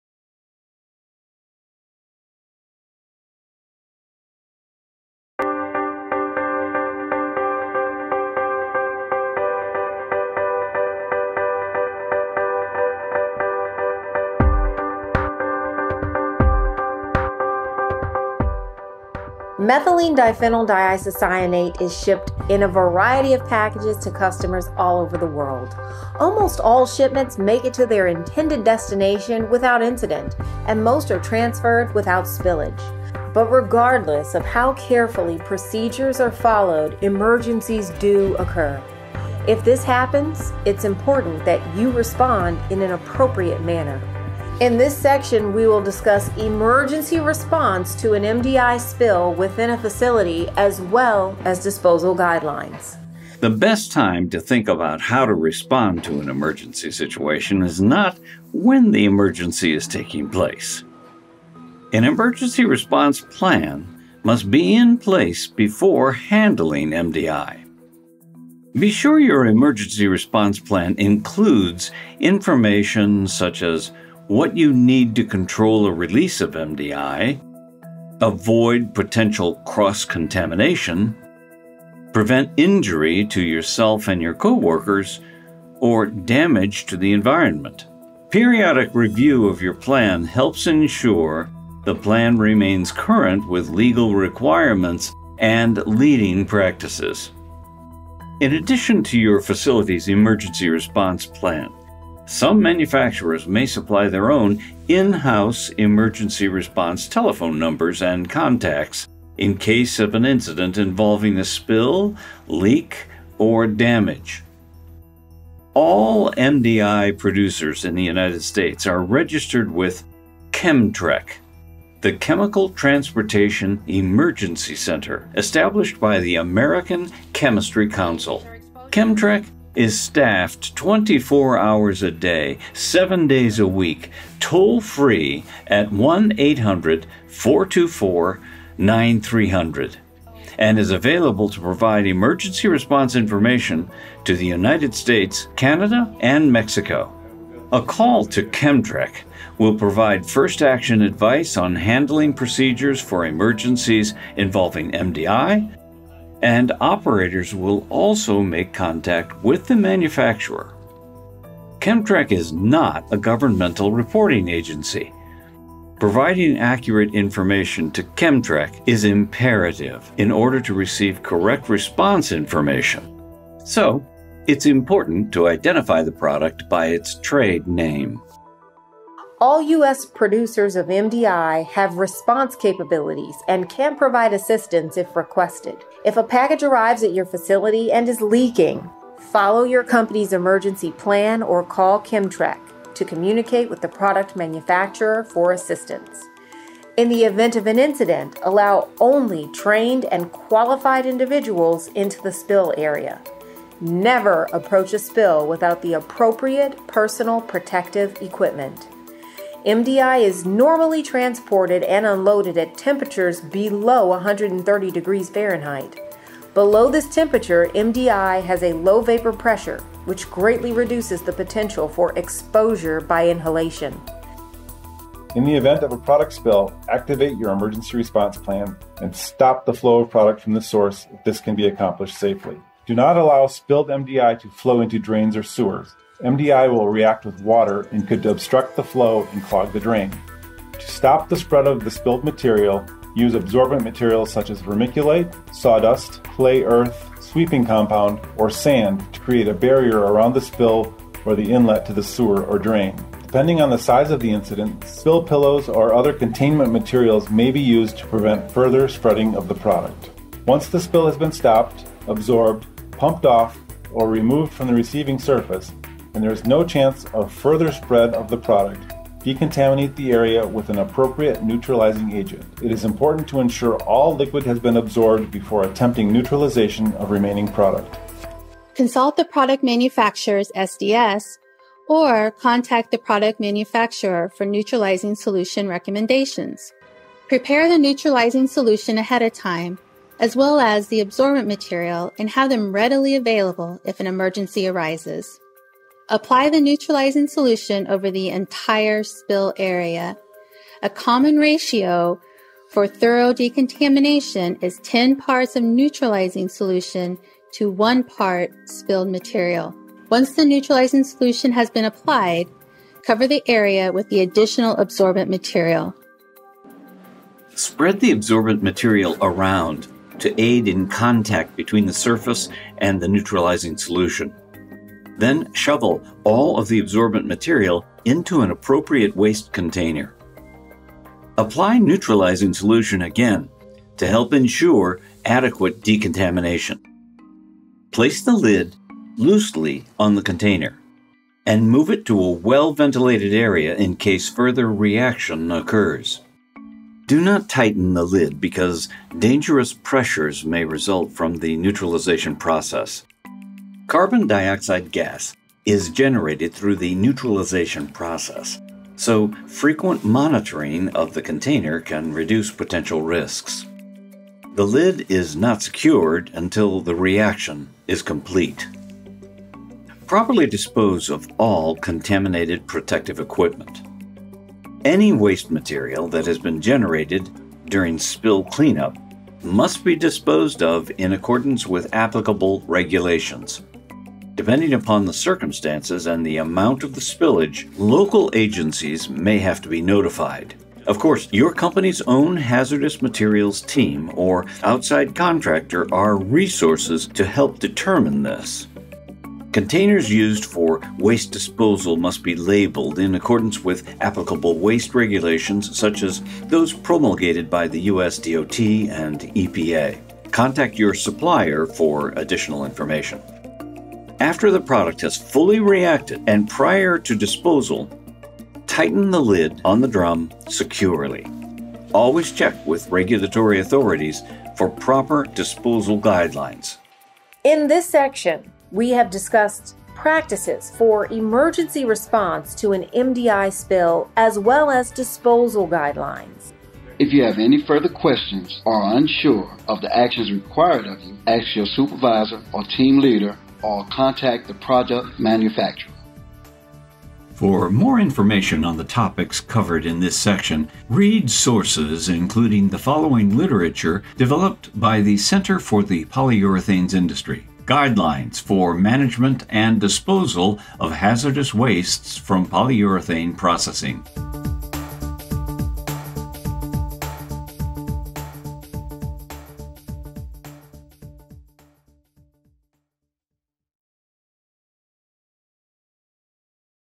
MDI Video - Part Six - Emergency Response
The Safe Handling of Methylene Diphenyl Diisocyanate (MDI) is a training program developed to inform employers and employees of industry best practices for the handling and transportation of MDI to minimize potential hazards. Topics included in the video series include hazard communication, personal protective equipment, and emergency response procedures. This program also discusses the best practices for drum or intermediate bulk container handling and unloading MDI from tank containers (isotainers), cargo tank trailers, and rail tank cars.